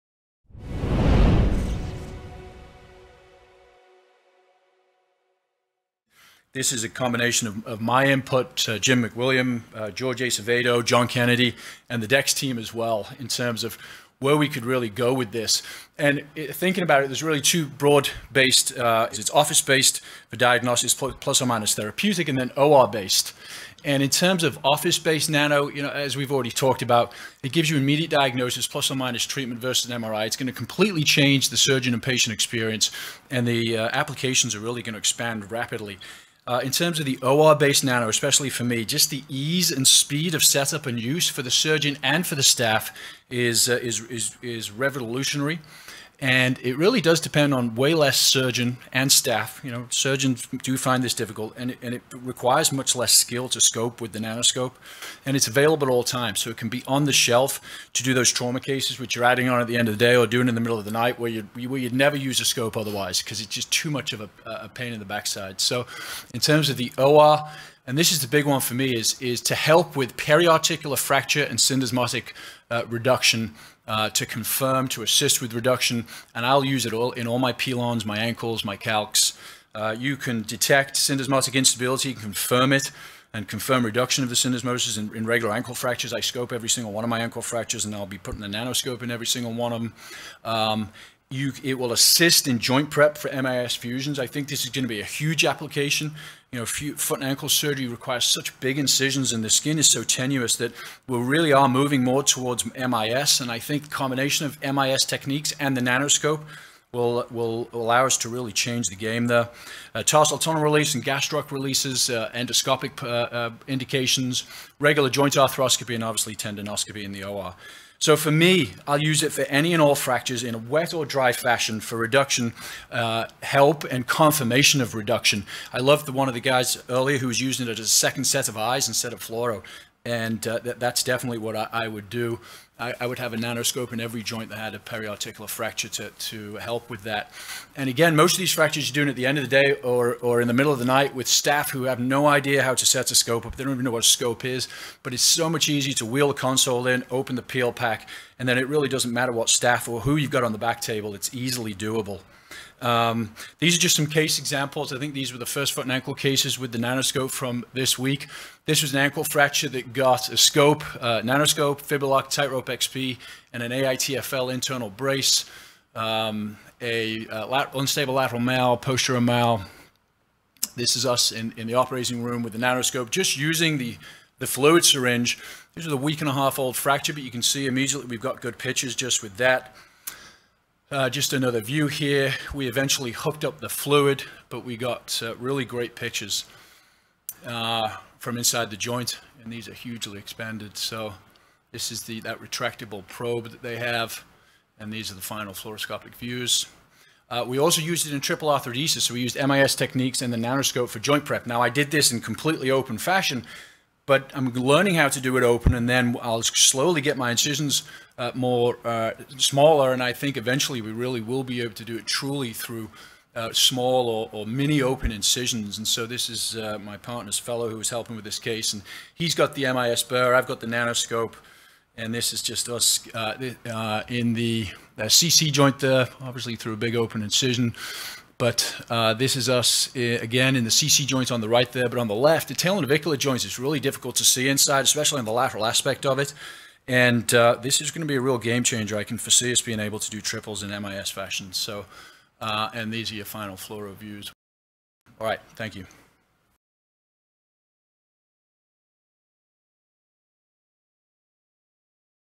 Ankle Nano Arthroscopy Applications
Christopher Hodgkins, MD (Miami, FL), discusses applications for NanoScope™ operative arthroscopy in the foot and ankle in this Train the Trainer presentation.